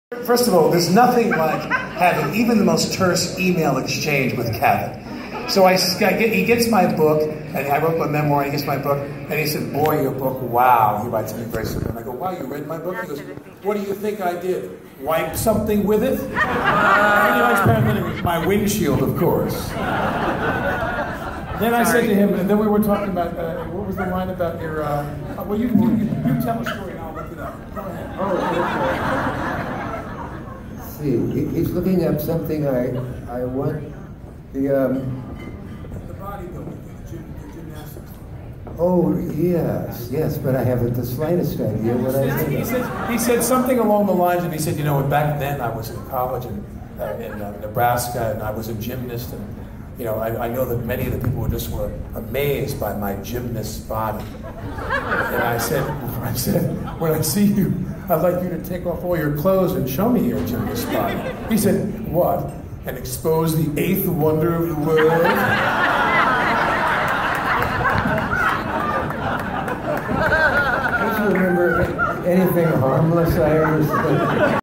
Alec Baldwin and the witty Dick Cavett at HIFF party
Alec gave Dick an award. Comnents before